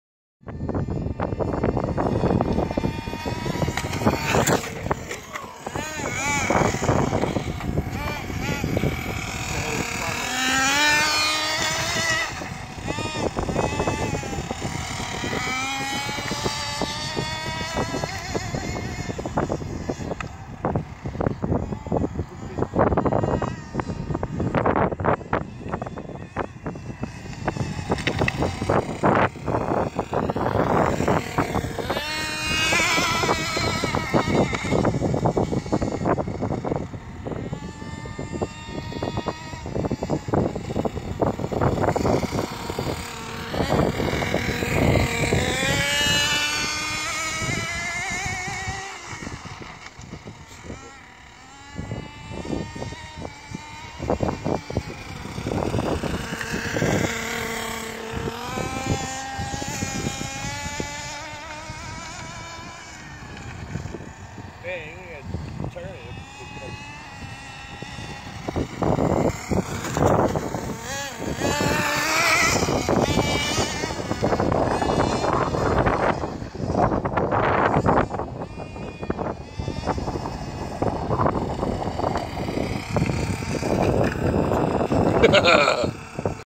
Johns nitro RX7
John running his new street nitro